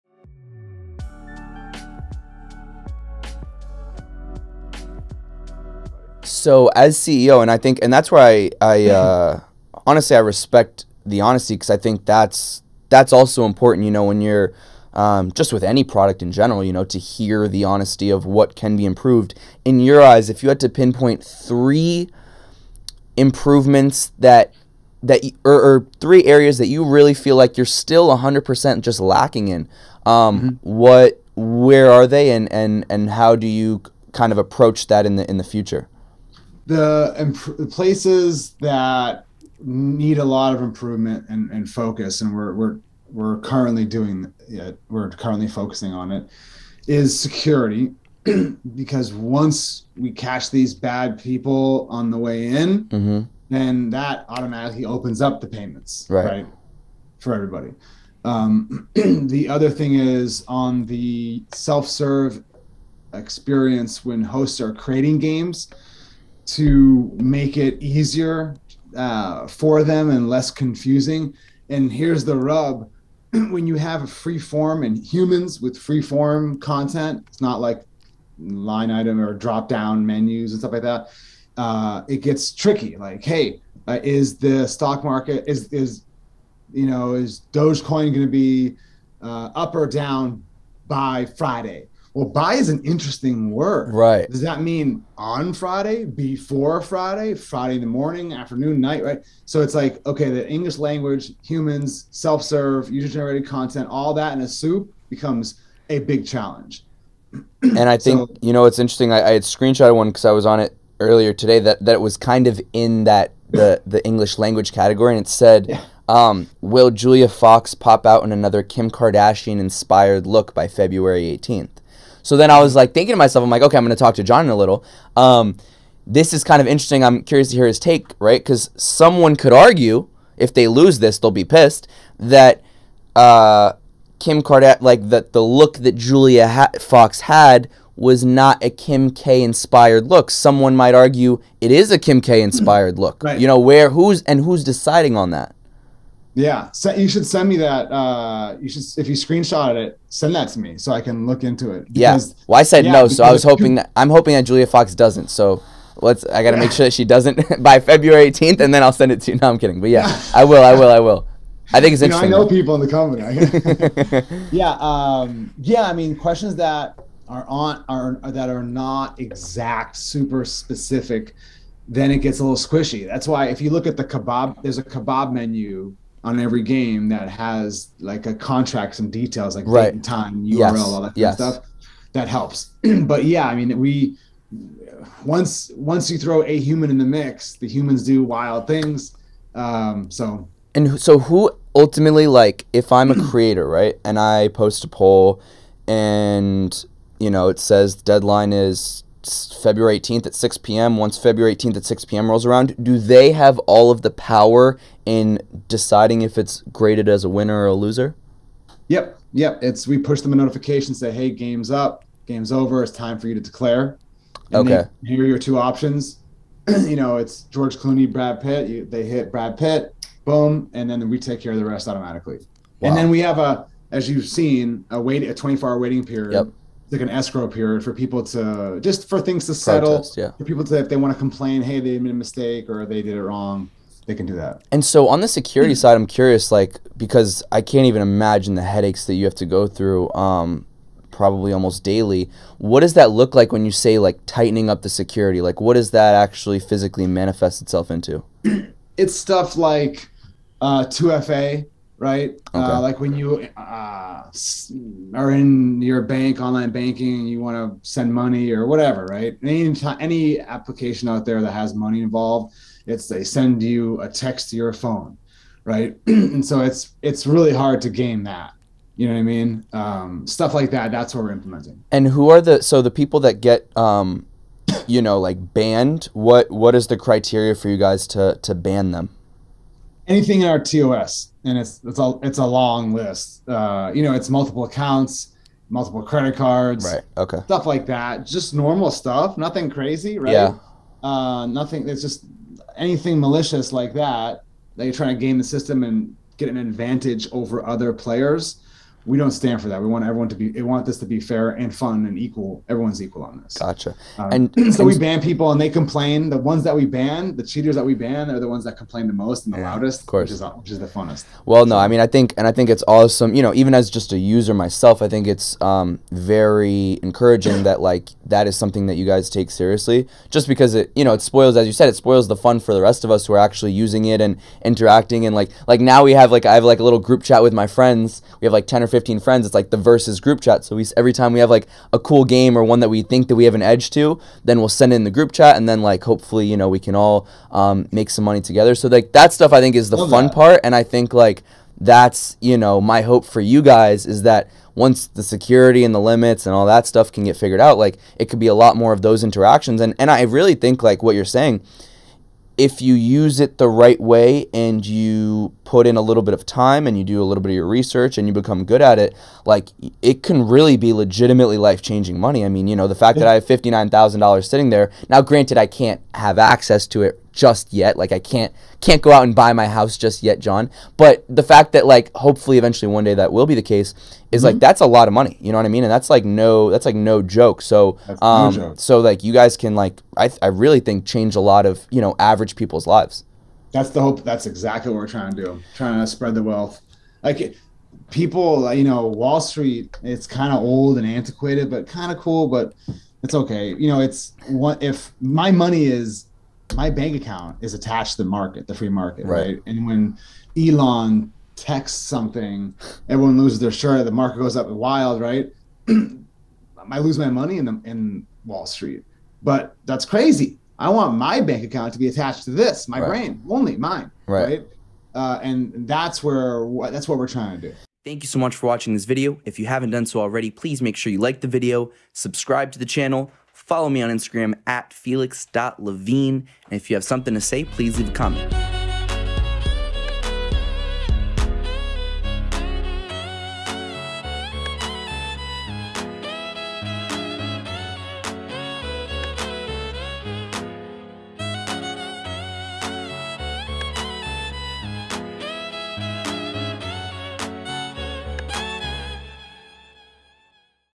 The Evolution of VersusGame | Where's This Going Podcast
John Vitti talks about the evolution of VersusGame and where he sees he can grow the app to give users a better experience.

 Or listen to the audio version ▶️